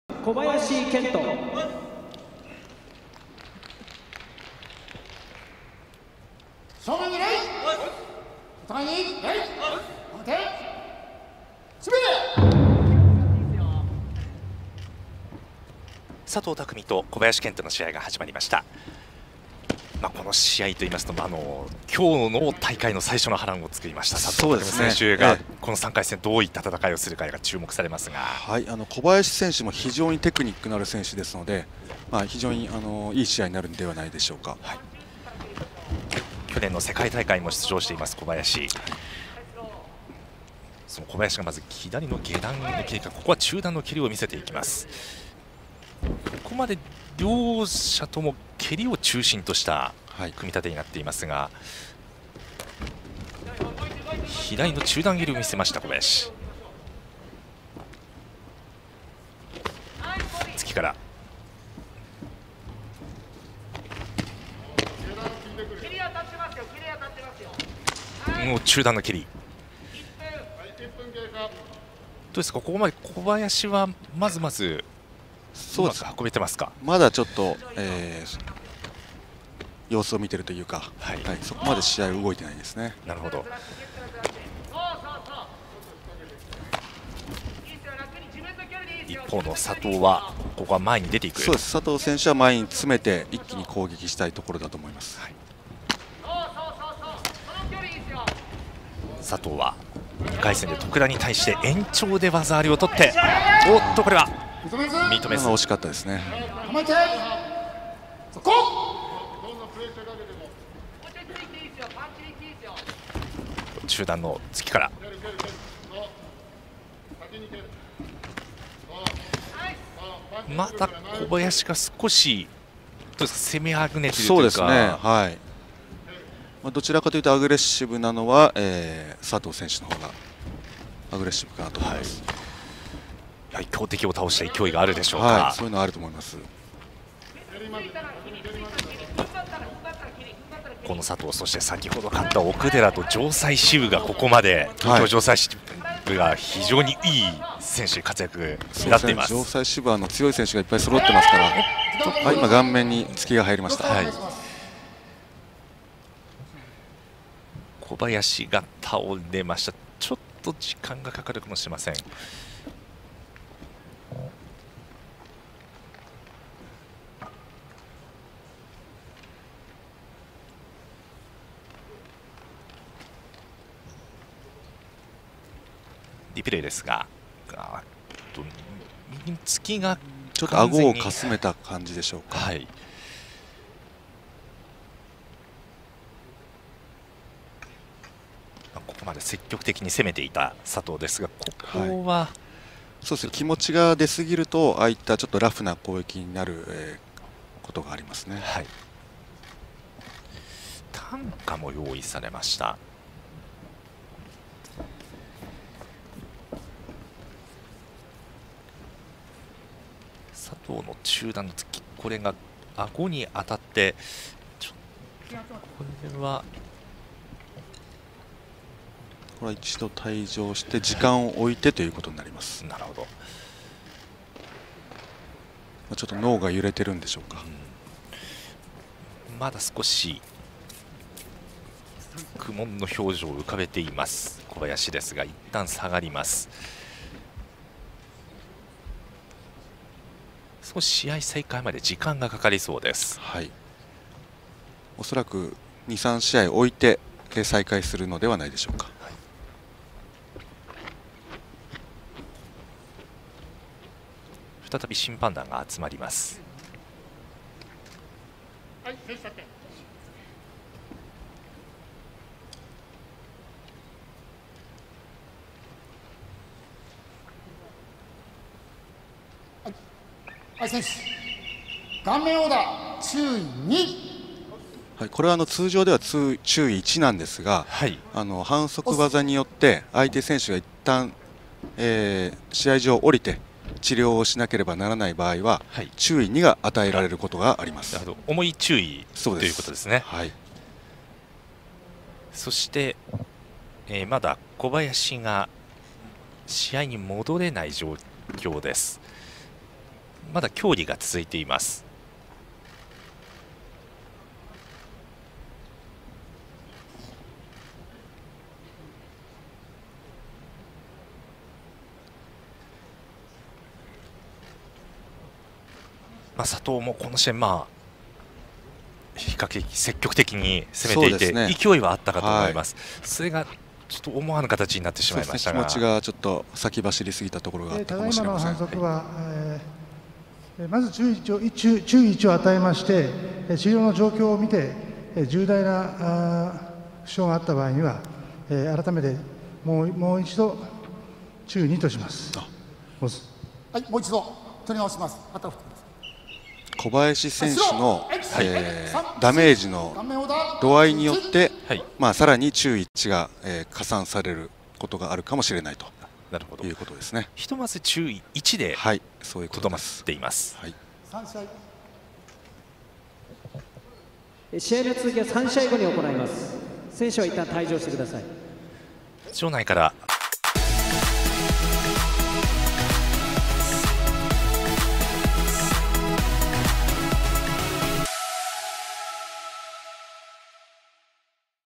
小林健人 VS 佐藤拓海【Kento Kobayashi VS Takumi Sato】
2020年 第52回全日本空手選手権 3回戦【2020 52nd All Japan Karate Championship Round 3】

世界各地で行われている空手の試合動画です。
This is a video of karate games being held all over the world.